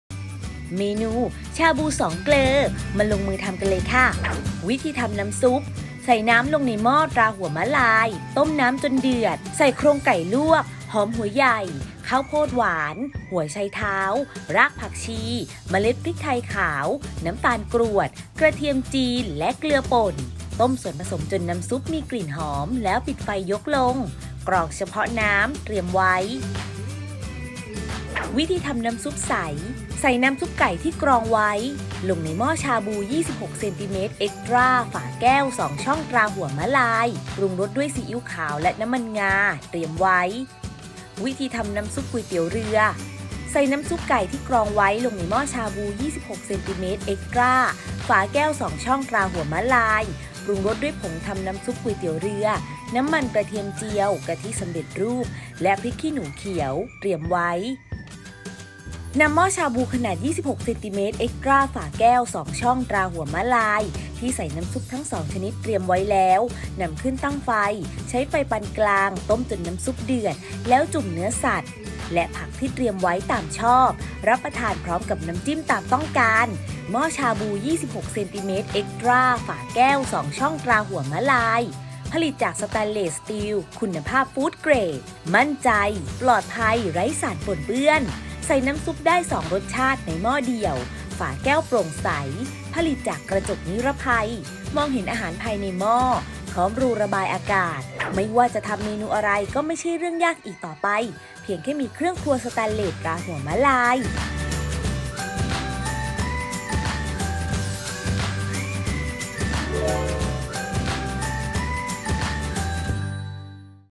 ชาบู2เกลอ สูตรเด็ด ทำง่าย สไตล์ "อ.ยิ่งศักดิ์"
ชาบู2เกลอ
2 style shabu

ผลิตภัณฑ์ : หม้อชาบู ขนาด 26 ซม. Extra ฝาแก้ว 2 ช่องตราหัวม้าลาย

สิ่งที่ต้องเตรียมสำหรับ (น้ำซุป)

โครงไก่สับเป็นชิ้นลวกสุก  1 ตัว
หอมหัวใหญ่ ผ่า 4 ส่วน  100 กรัม
ข้าวโพดหวาน หั่นเป็นท่อน  100 กรัม
หัวไชเท้าปอกเปลือกหั่นเป็นท่อน หนาประมาณ 1 ซม.  100 กรัม
รากผักชีบุบพอแตก  30 กรัม
เมล็ดพริกไทยขาว  1 ช้อนโต๊ะ
น้ำตาลกรวด  20 กรัม
กระเทียมจีน  1 หัว

1.ใส่น้ำลงในหม้อ ตราหัวม้าลาย ต้มน้ำจนเดือด เติม โครงไก่ลวก หอมหัวใหญ่ ข้าวโพดหวาน หัวไชเท้า รากผักชี เมล็ดพริกไทยขาว น้ำตาลกรวด กระเทียมจีน และเกลือป่น  ต้มส่วนผสมจนเดือดใช้ไฟปานกลาง จนส่วนผสมน้ำซุปมีกลิ่นหอมปิดไฟยกลงกรองเฉพาะน้ำเตรียมไว้

สิ่งที่ต้องเตรียมสำหรับ (น้ำซุปใส)

น้ำซุปไก่ที่กรองเตรียมไว้ 1.5 ลิตร

1.ใส่น้ำซุปไก่ที่กรองไว้ลงในหม้อ ชาบู 26 ซม. Extra ฝาแก้ว 2 ช่องตราหัวม้าลาย
ปรุงรสด้วยซีอิ๊วขาวและน้ำมันงา เตรียมไว้

สิ่งที่ต้องเตรียมสำหรับ (น้ำซุปก๋วยเตี๋ยวเรือ)

น้ำซุปไก่ที่กรองเตรียมไว้  1 ลิตร
ผงทำน้ำซุปก๋วยเตี๋ยวเรือ       3 ช้อนโต๊ะ
น้ำมันกระเทียมเจียว      2 ช้อนโต๊ะ
พริกขี้หนูสวนเขียวโขลกละเอียด     2 ช้อนโต๊ะ

1.ใส่น้ำซุปไก่ที่กรองไว้ลงในหม้อชาบู 26 ซม. Extra ฝาแก้ว 2 ช่องตราหัวม้าลาย
ปรุงรสด้วยผงทำ น้ำซุปก๋วยเตี๋ยวเรือ น้ำมันกระเทียมเจียว
กะทิสำเร็จรูปและพริกขี้หนูเขียวเตรียมไว้

สิ่งที่ต้องเตรียมสำหรับ (ส่วนผสมชาบู)

เนื้อหมูสำหรับชาบูสไลด์บาง  500 กรัม
เบคอน 200 กรัม
น้ำมันงา สำหรับหยอดหน้าหมู  3 ช้อนโต๊ะ
งาขาวคั่วบุบพอแตกสำหรับโรยหน้า
ผักสด ผักกาดขาว ต้นหอมญี่ปุ่น เห็ดออรินจิ เห็ดเข็มทอง
เห็ดชิเมจิดำขาว ผักกวางตุ้งไต้หวัน แครอท กดด้วยพิมพ์ดอกไม้
ฟักทอง กดด้วยพิมพ์ดอกไม้ และกะหล่ำปลีซอยฝอย
วุ้นเส้น หรือ ก๋วยเตี๋ยวเส้นเล็ก ตามชอบ
น้ำจิ้มสุกี้สูตรที่ชอบ
ผักชี ซอยละเอียด พริกขี้หนูสับ
และกระเทียมสับ และน้ำมะนาวสำหรับเพิ่มรสชาติ

1.นำหม้อชาบู ขนาด 26 ซม. Extra ฝาแก้ว 2 ช่องตราหัวม้าลาย ที่ใส่น้ำซุปทั้ง 2 ชนิดเตรียมไว้แล้ว
2.นำขึ้นตั้งไฟ ใช้ไฟปานกลาง ต้มจนน้ำซุปเดือด จุ่มเนื้อสัตว์ และผักที่เตรียมไว้ตามชอบ รับประทานพร้อมกับน้ำจิ้มตามต้องการ

▲Line @yingsakfood